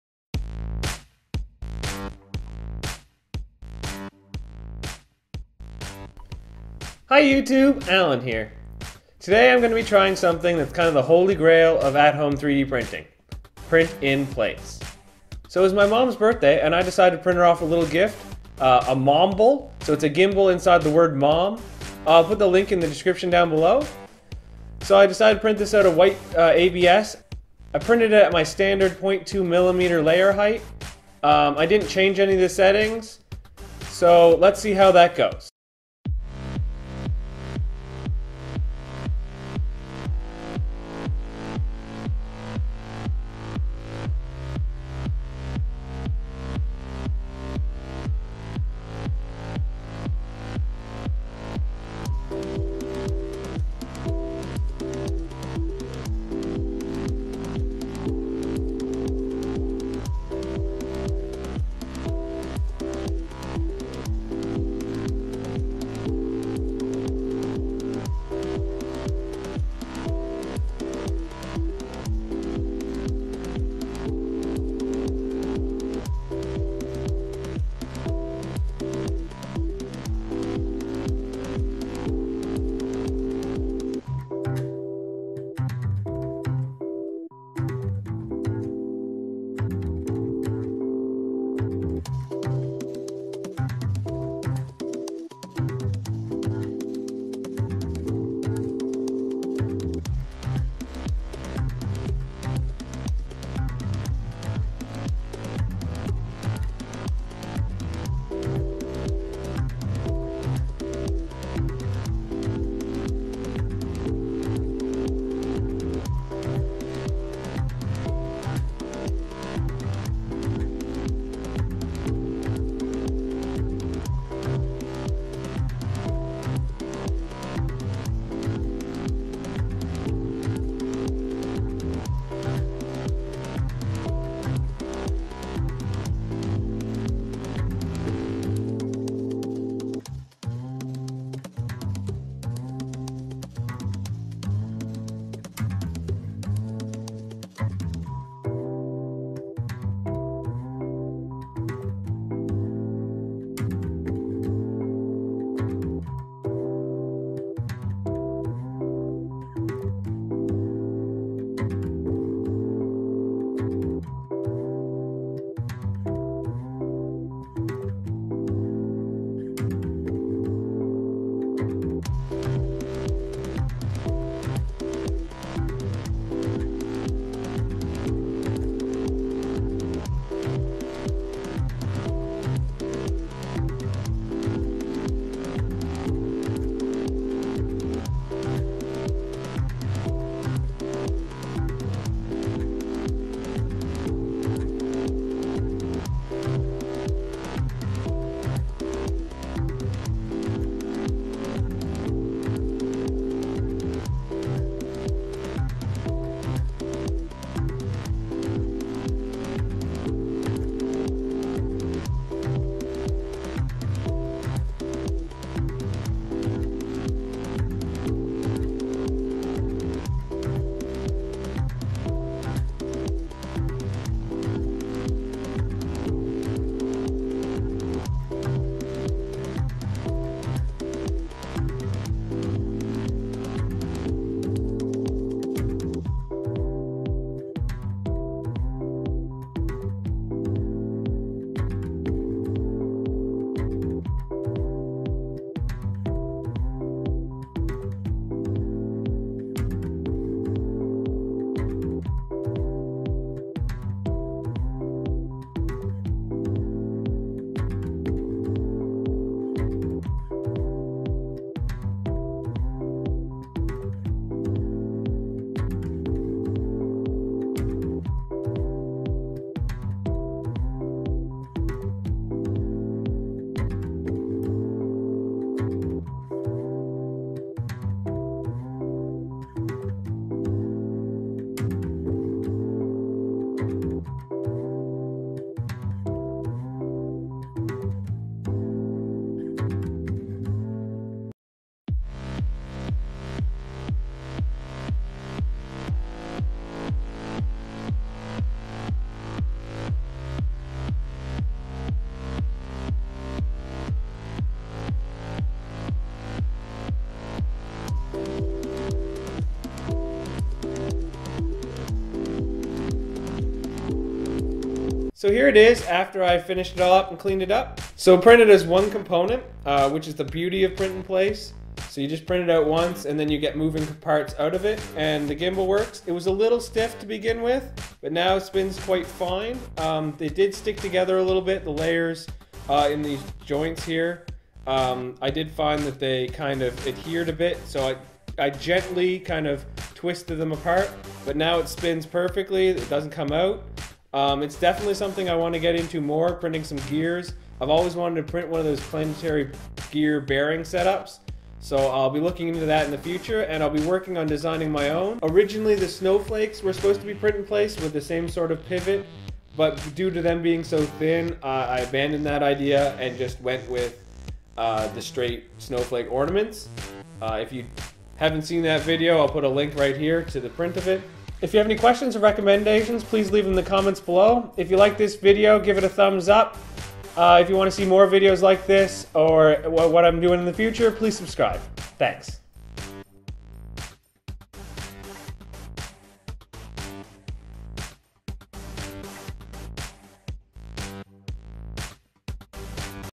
Cool Printing: Print in Place
Hey Youtube,

Printing in place is kinda the holy grail of home 3D printing and is something I have wanted to try for a while. but I do want to work on creating my own designs so stay tuned.

If you have any suggestions for what I should do next leave them in the comments below.

Cheers!